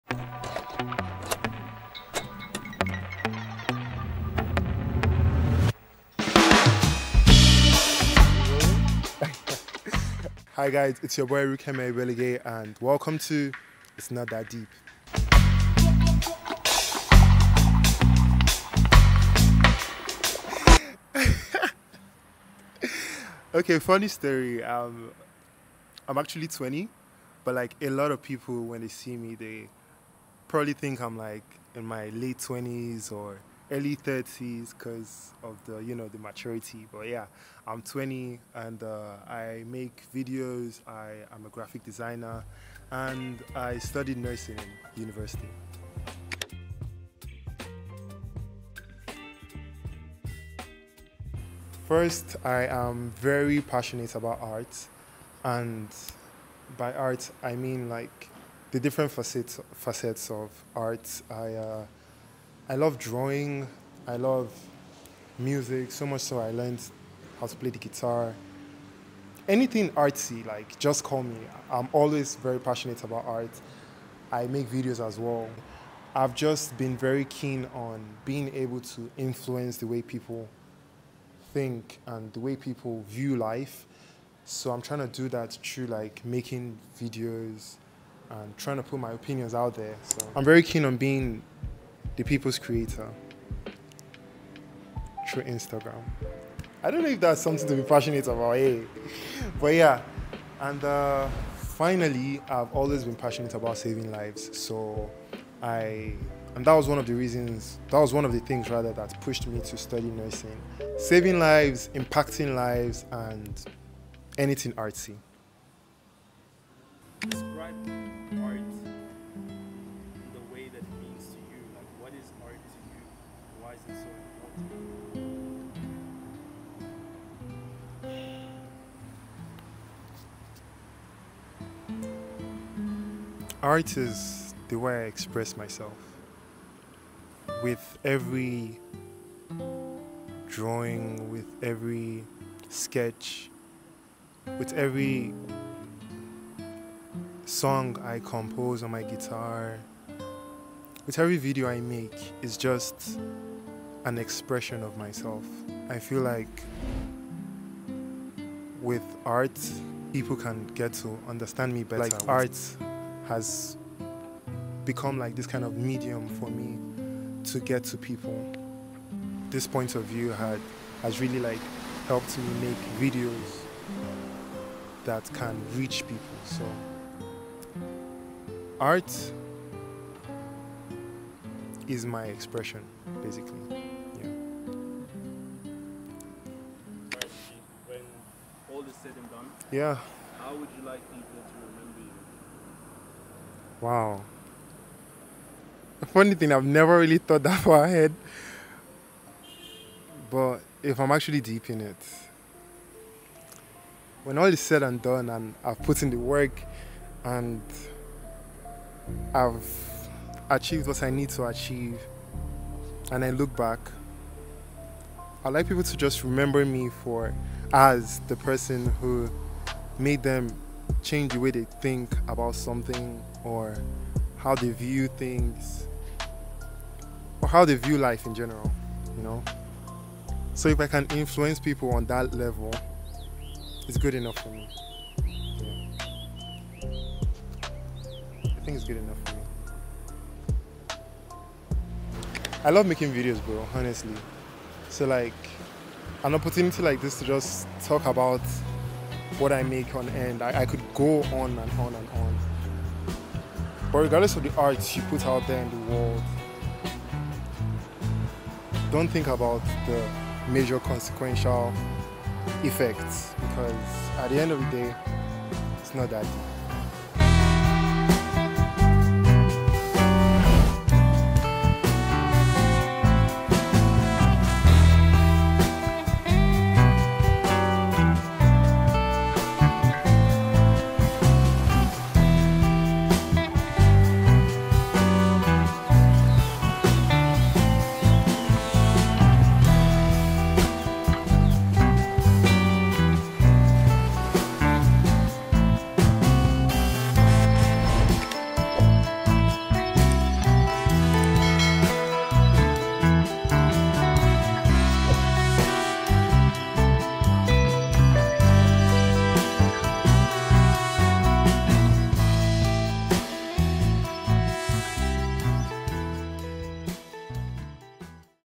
It’s Not That Deep | Cassette 01 | Rukeme Egbelughe
Hey Family,

As yous know (if you didn’t, well, now you know) I’m proper passionate about making documentaries where we get to tell a story from someone else’s perspective. I find that to be so fulfilling and exciting to bring their words to reality as best as I envisioned it in me head.

In this series, yous get about 5 minutes to hear from a new person- what they do, how they view life and , as you’ll find out, soon enough- how they want to be remembered!

I hope some of my questions gets you thinking a bit and if they help you in any way, please do let me know, I’d love to engage in more discourses…and if it did nothing but (maybe…hopefully) entertain- then that’s okay too.

At the end of the day, whatever you do, remember…

It’s Not That Deep!

Until next time, know that i'm so grateful to you for having taken the time out your day to watch this video- Merci beaucoup!

Love,
Joël

Chapters:

0:00    Intro
0:17    Question One
0:46   Question Two
1:57    Question Three
3:07   Question Four
4:02   Outro